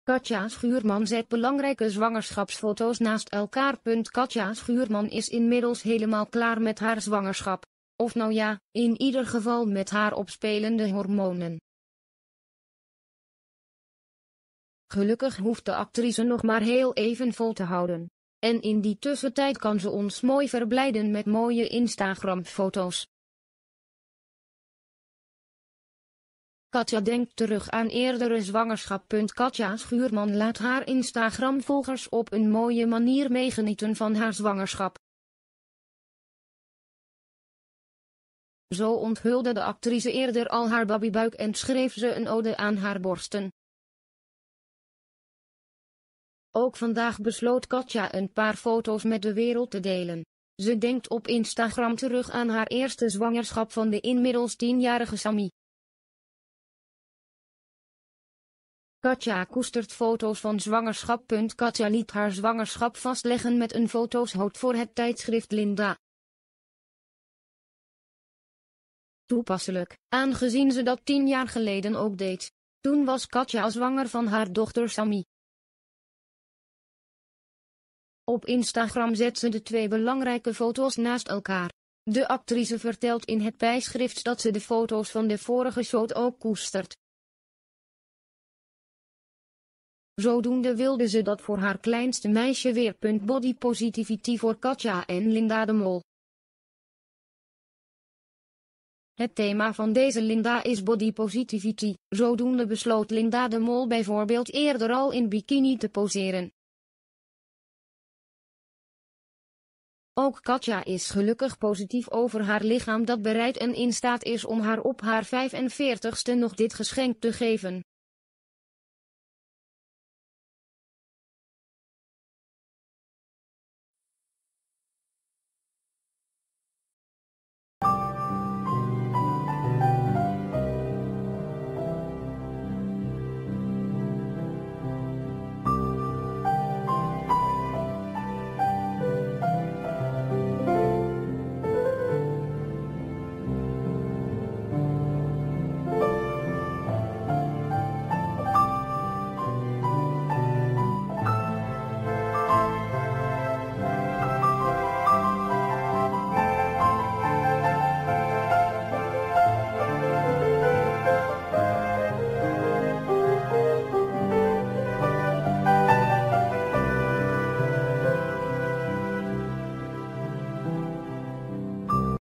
Katja Schuurman z*e*t be|@ngrijke zw@ngerschapsf0t0’s n@@st e|kaar.
Katja Schuurman zet belangrijke zwangerschapsfoto’s naast elkaar.
Katja Schuurman is inmiddels helemaal klaar met haar zwangerschap! Of nou ja, in ieder geval met haar opspelende hormonen. Gelukkig hoeft de actrice nog maar heel even vol te houden. En in die tussentijd kan ze ons mooi verblijden met mooie Instagram-foto’s!
Katja denkt terug aan eerdere zwangerschap.
Katja Schuurman laat haar Instagram-volgers op een mooie manier meegenieten van haar zwangerschap.